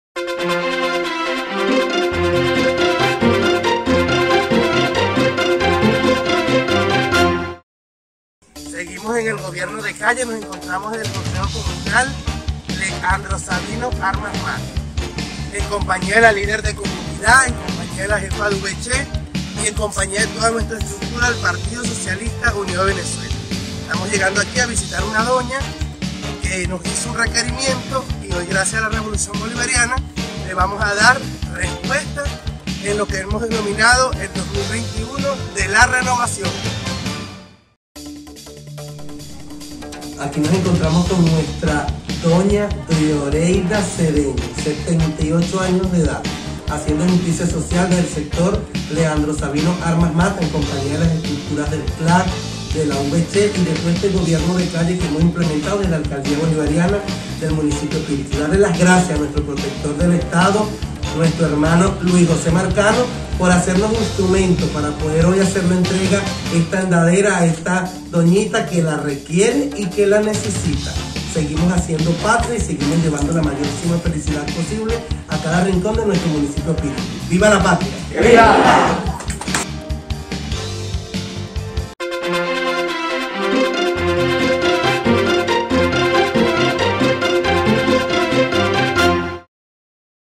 La Alcaldía Bolivariana de Píritu inició el Año 2021 con el Gobierno de calle
La Alcaldíaldía Bolivariana del municipio píritu en la zona oeste del Estado anzoátegui a través de la gestión del alcalde Williams Petit continua llevando bienestar a nuestros adultos mayores.